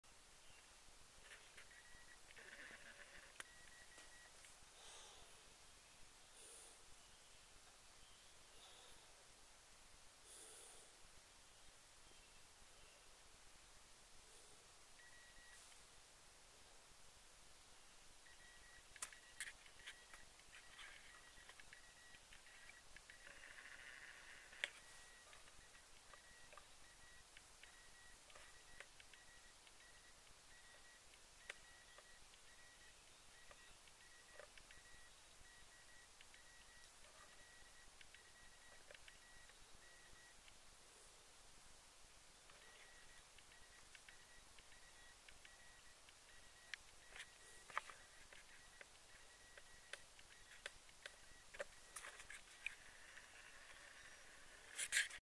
Zajączek - koniec śniadania
Zajączek kończy śniadanie. Kilka ziarenek na deser.